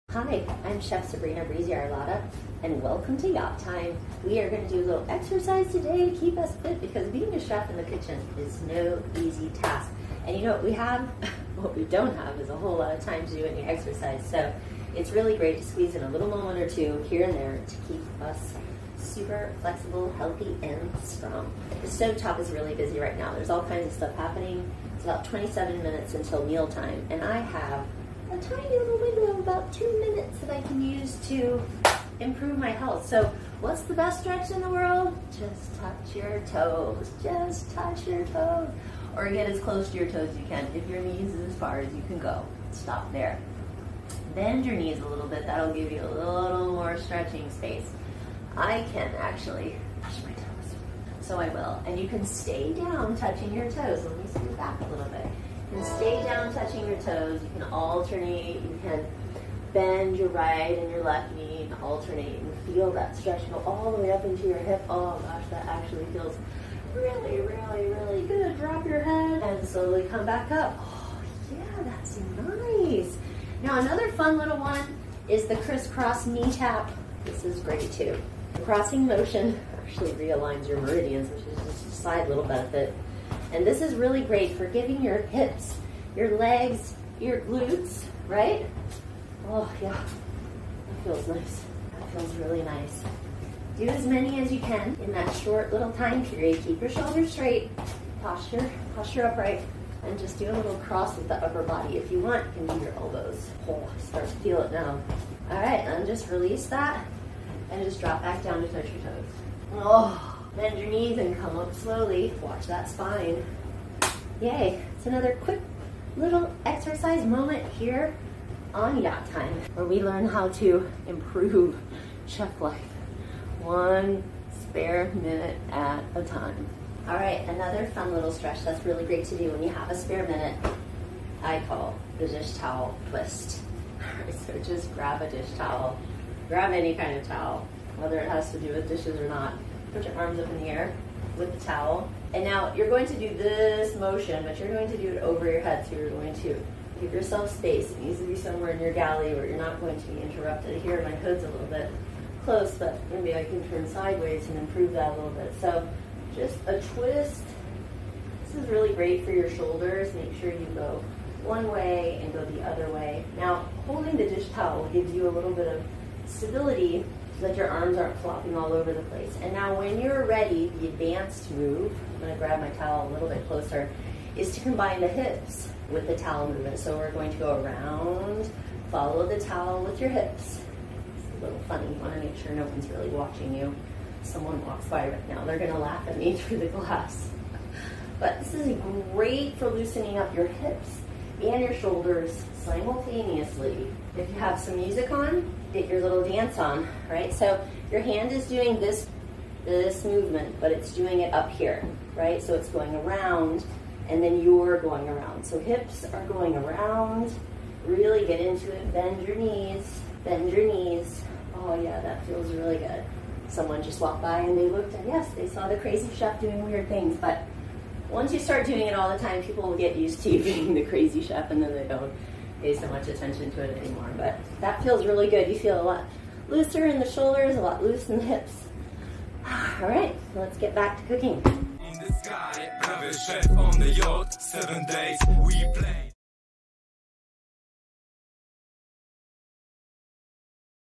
Superyacht Chef Workout | Dishtowel Twist
Just a few minutes a day to a healthier BACK, stronger LEGS, and LESS PAIN in your body.

Hello! I am Chef Sabrina Breezy Arlotta and welcome to yacht time! I am here today to give you a few ideas to help you squeeze in a little workout to balance your body and reduce your body pains. Working as a superyacht chef is physically demanding. Long days and nights cooking for owners, guests and crew can leave your body feeling intense pain from your neck to your toes.

Using these helpful hints, mini workouts, and simple motions while doing every day galley and kitchen tasks, will improve your body's flexibility, break up the monotony of a busy day on yacht, and strengthen your muscles.  Immediately you will feel the benefits of taking a little time to nourish your chef body. Now join me as we explore some simple exercises and form some new habits with a few fun galley tips of the trade.

Healthier chefs make more delicious food! Enjoy!